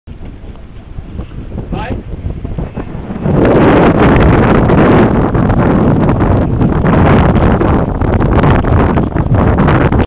Zweefvliegen deel 5
In mei 2007 heb ik als passagier gevlogen in een zweefvliegtuig in de buurt van Surwold Duitsland.
Het was interessant, maar ik was zo misselijk! Dat is te horen aan dat gezucht en gesteun van mij.